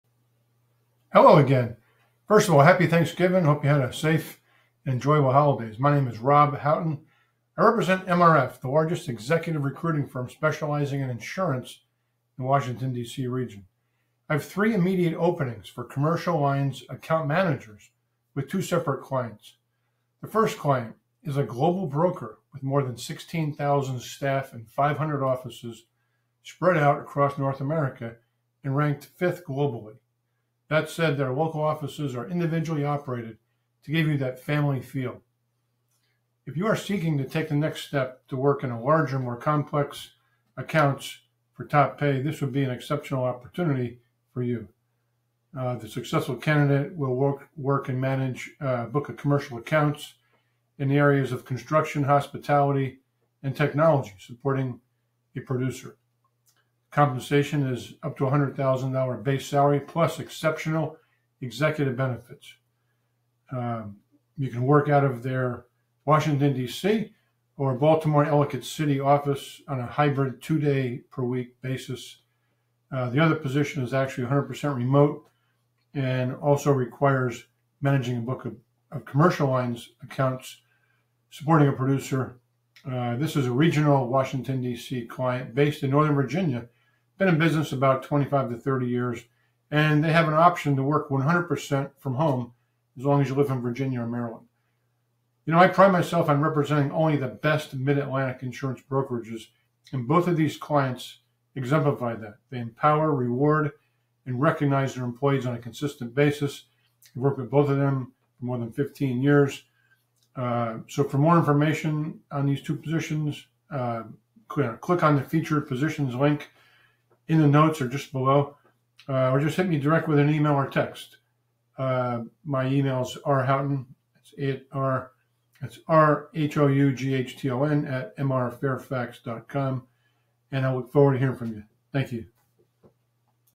Wanted: Three Commercial Lines Account Manager (Remote)
Commercial Lines Positions:
Position#1:
Location: Baltimore/Ellicott City or WDC
Position Title: Commercial Lines Account Manager
Work Environment: Hybrid (Two Days per week in office)
Compensation: Up to $100k in salary
Benefits:  Executive Level
Experience:  Large accounts in the construction, hospitality, tech sectors

Position #2
Location: Northern Virginia
Work Environment: 100% Remote
Compensation: Up to $90k in salary
Benefits:  Executive Level
Experience:  Medium-sized general commercial accounts